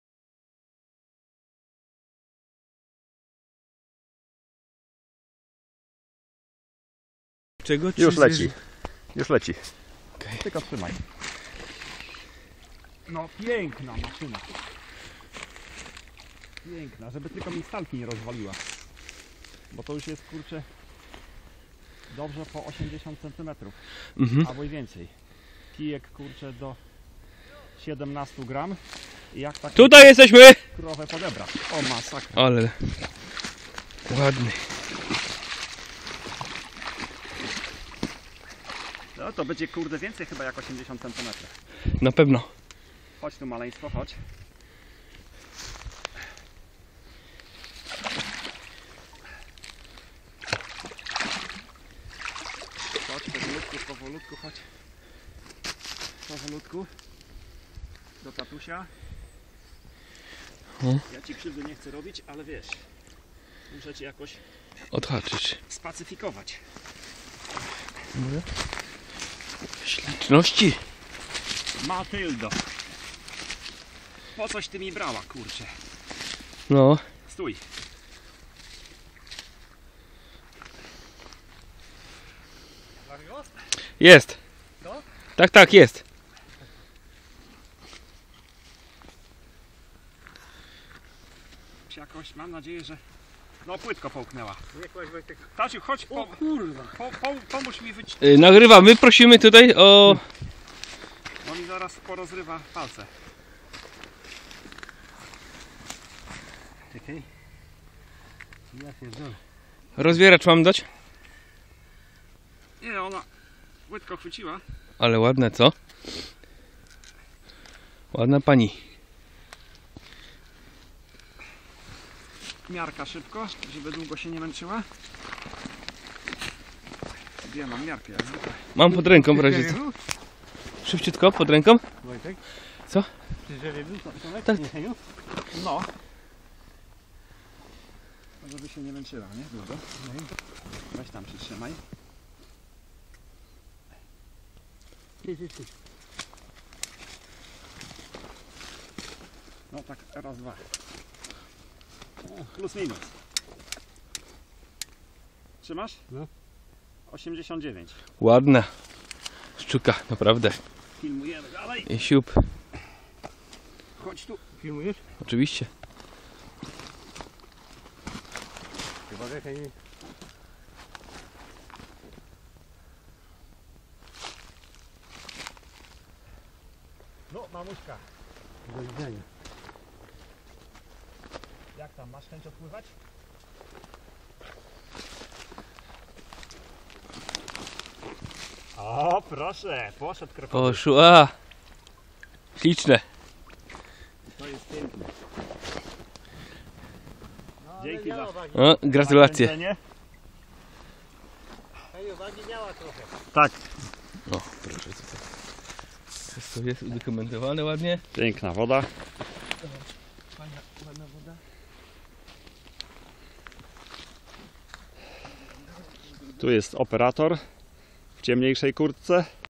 Zimno i biorą grube ryby.
Wystarczy że temperatury nocami spadną poniżej 5 st.C a grube drapieżniki uaktywniają się, czując zbliżającą się zimę.